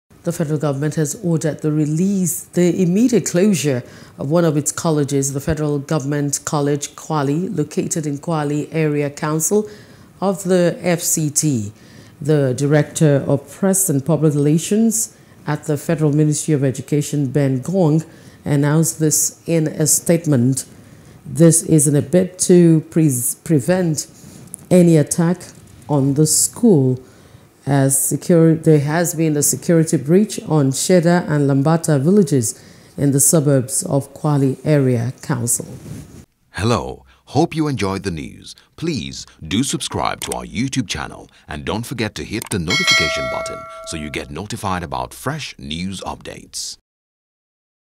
Abuja: F.G. Shuts F.G.C. Kwali Over Security Threat | NEWS
The Federal Government has ordered the immediate closure of one of its colleges, the Federal Government College, Kwali located in Kwali Area Council of the Federal Capital Territory, Abuja.